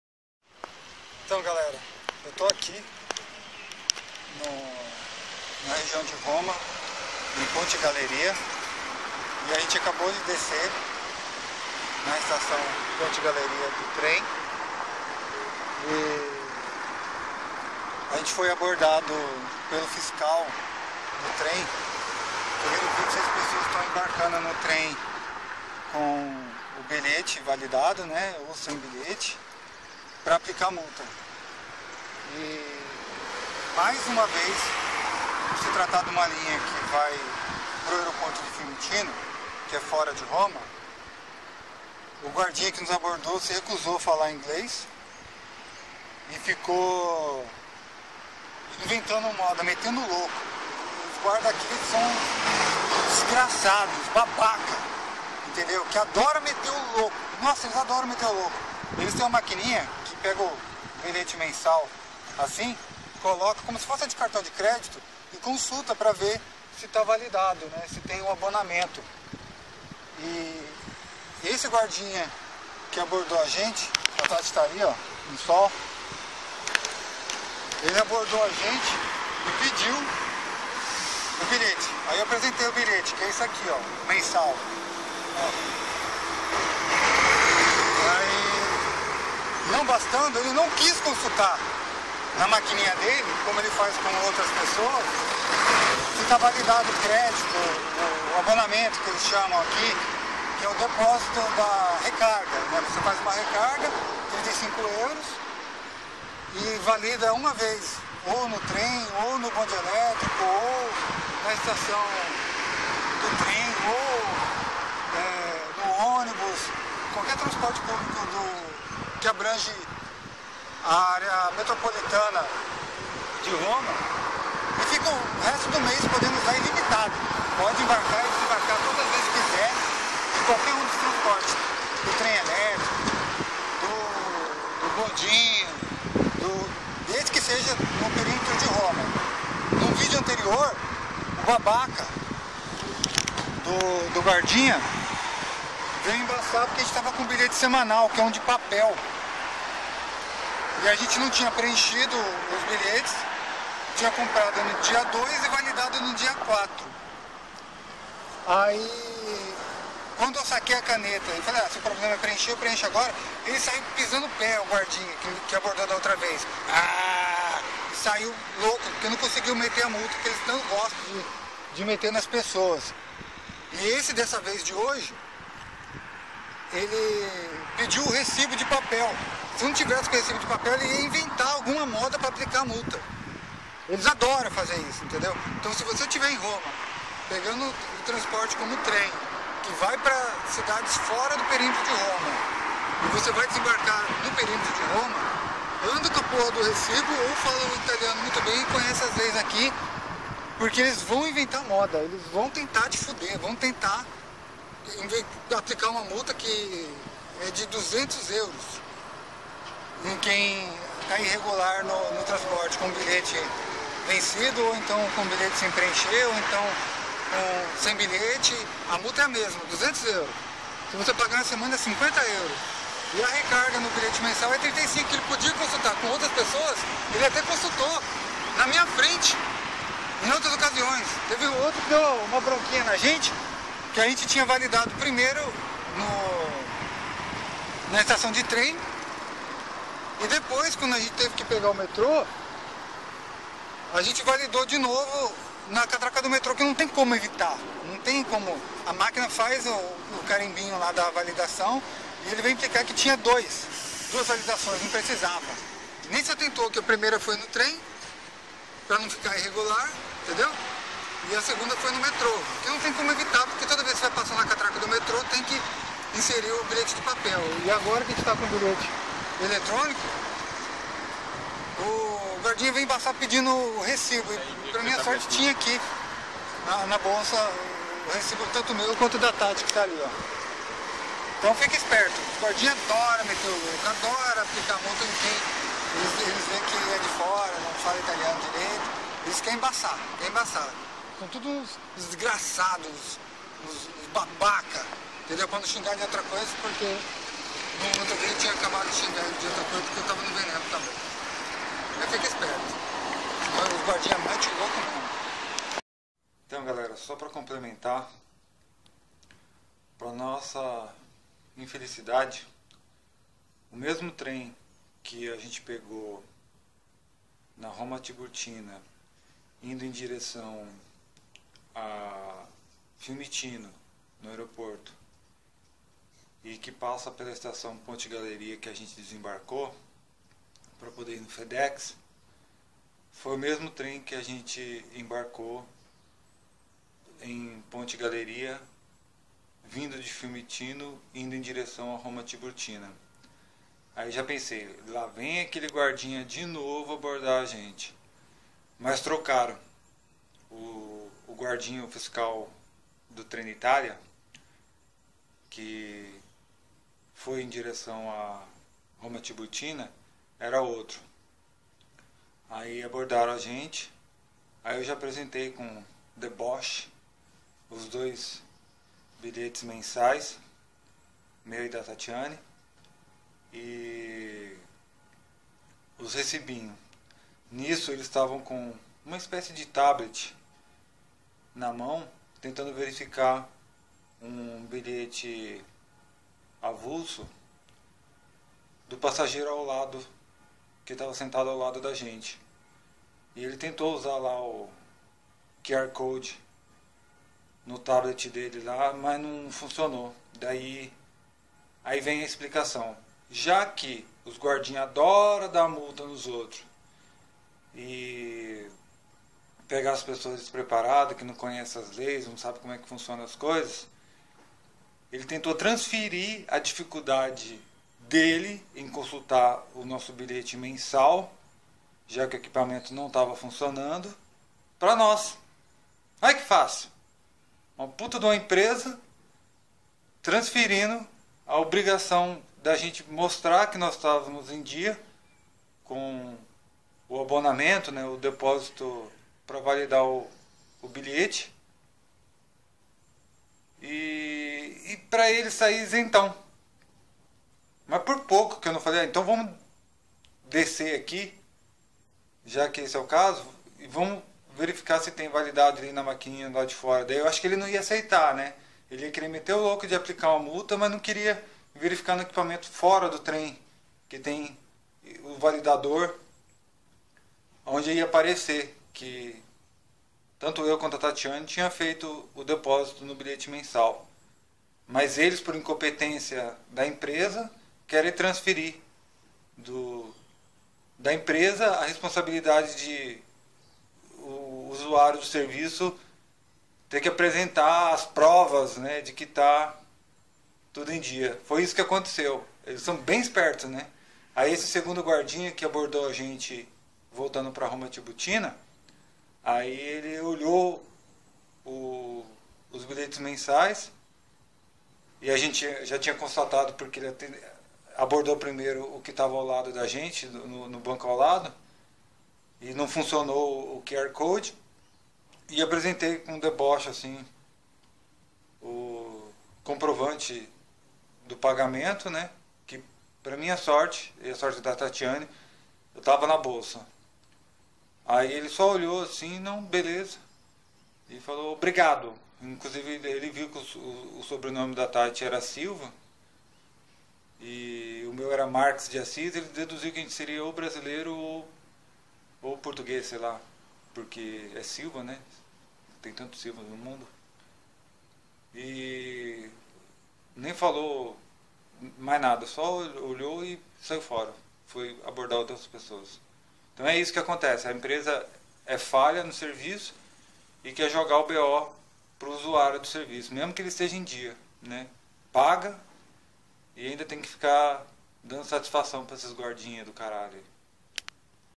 Mais uma abordagem do guardinha / fiscal da Trenitalia em Roma - Caminhada Pelo Mundo
Fique esperto com os guardinhas da Trenitalia! Eles metem o loko! Só que comigo não deu certo nenhuma vez.